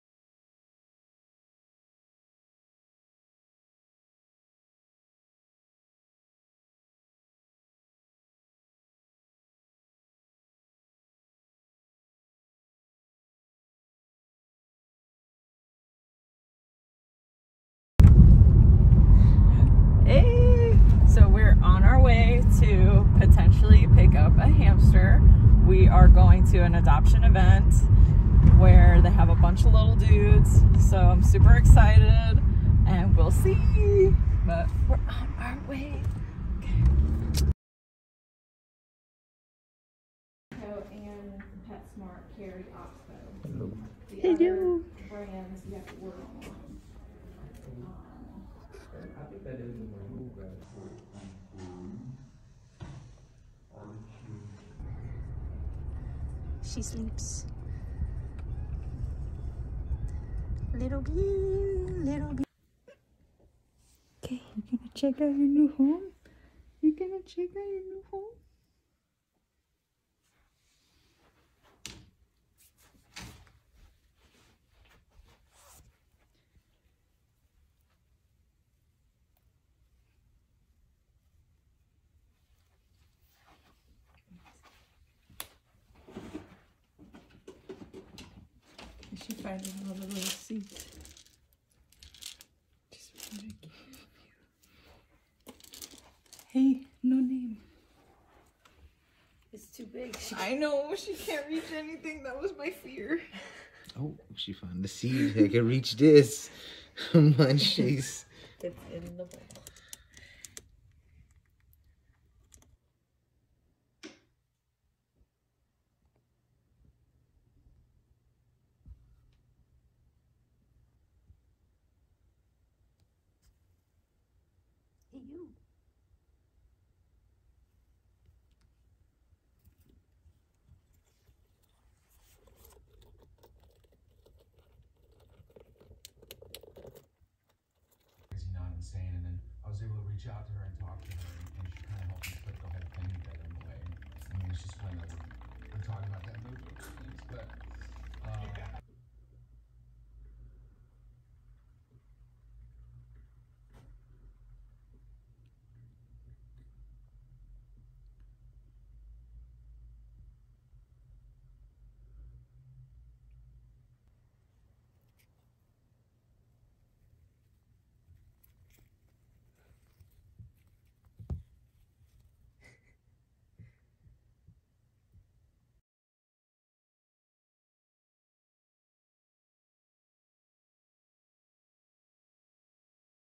Meet my New Hamster | I Adopted a Dwarf Hamster!
Hey everyone, thanks for clicking on my video! sorry some parts are a little wonky, the software I was using has recently started giving me problems, so now I need to look into a new one lol!

I hope you liked meeting Kimi, like I wrote in the video, she was adopted from Magic Happens Rescue.  This was such a professional place, I was able to apply for adoption and they made sure to ask what size and type of enclosure I had for her and they made sure all of her needs were going to be taken care of prior to the adoption event.

There are a lot of animals waiting to be adopted so if you are ready and looking for a new pet please consider adopting! This was such a great feeling knowing I was helping out a rescue and gaining a new member of the family all at the same time!